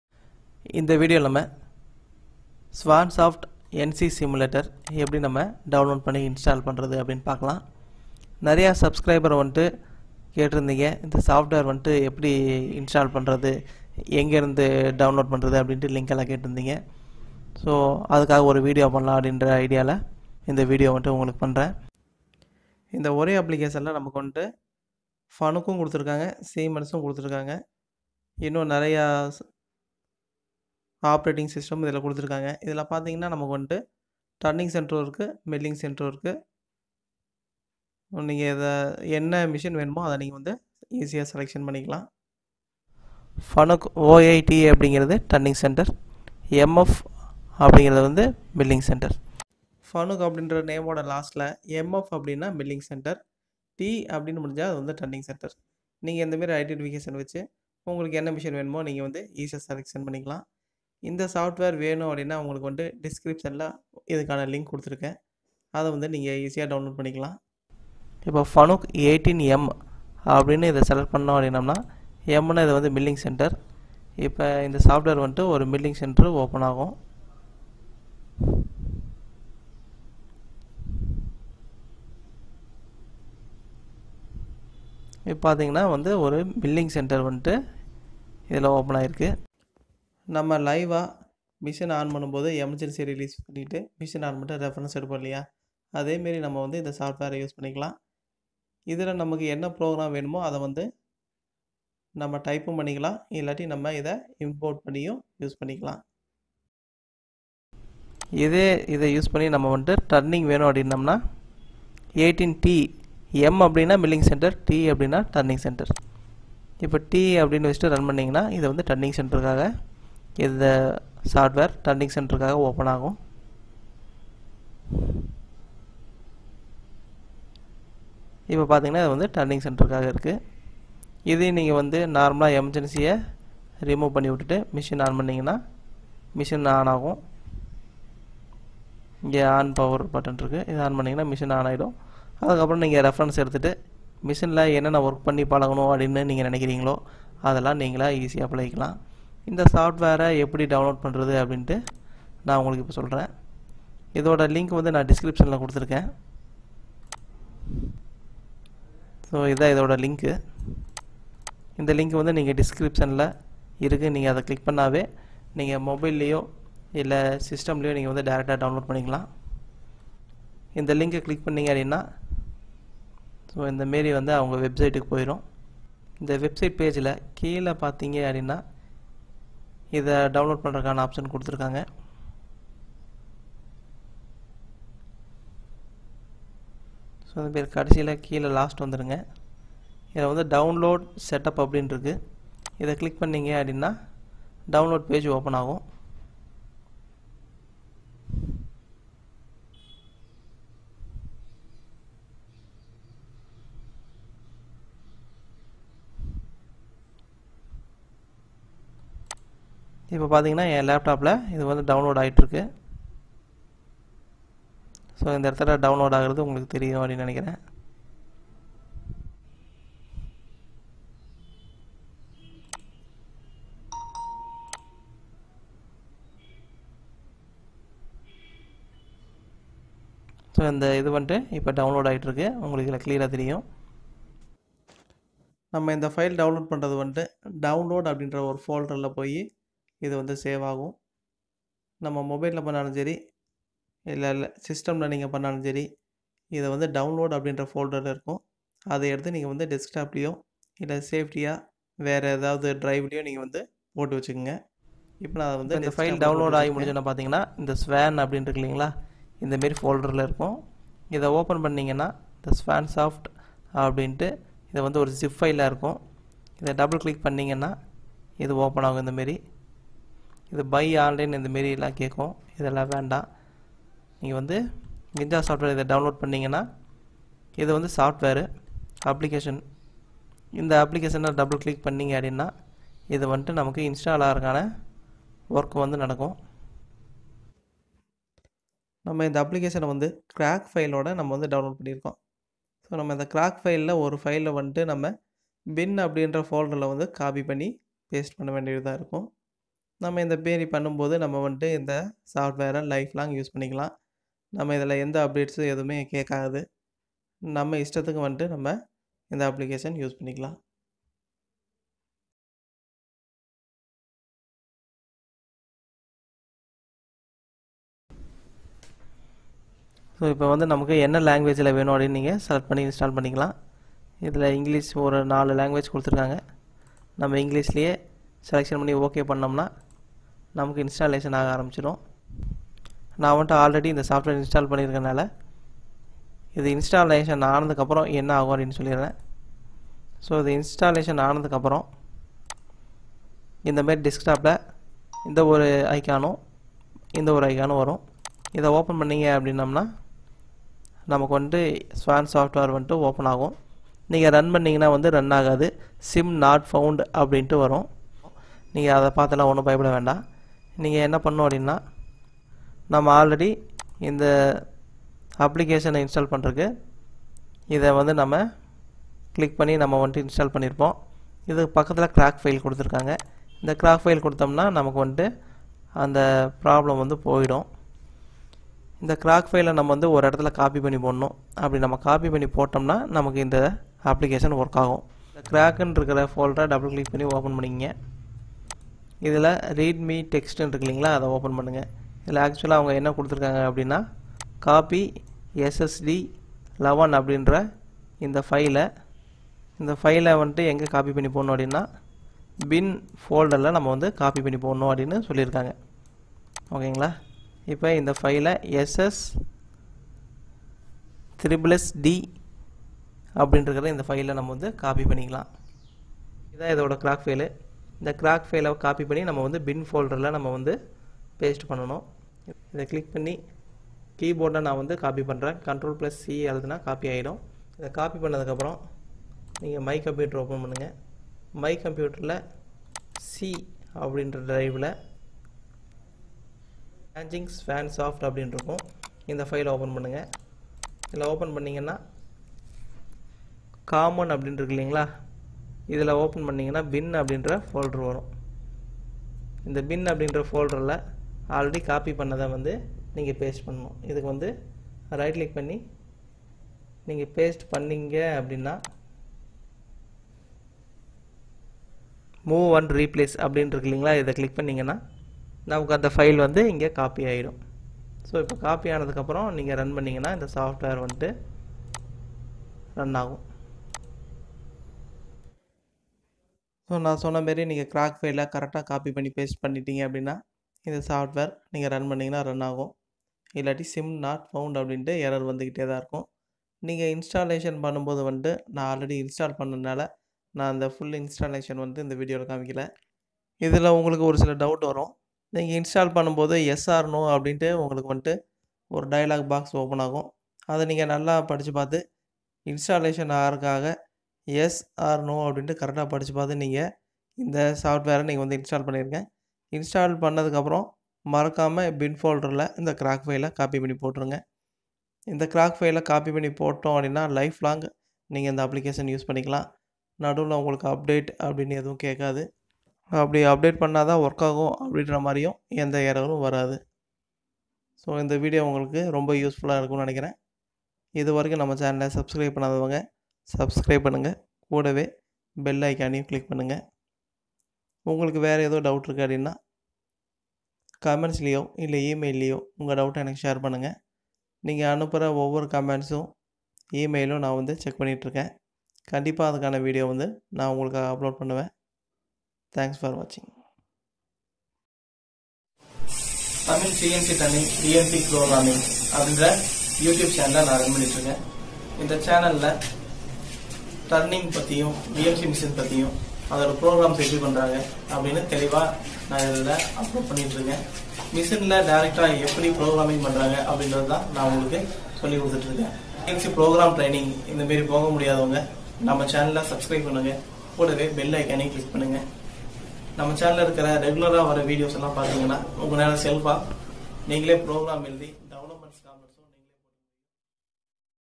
Swansoft CNC simulator free download and installation video in Tamil
I explained about how to download and install CNC swansoft CNC simulator in Tamil

This video is very useful for CNC PROGRAM learners.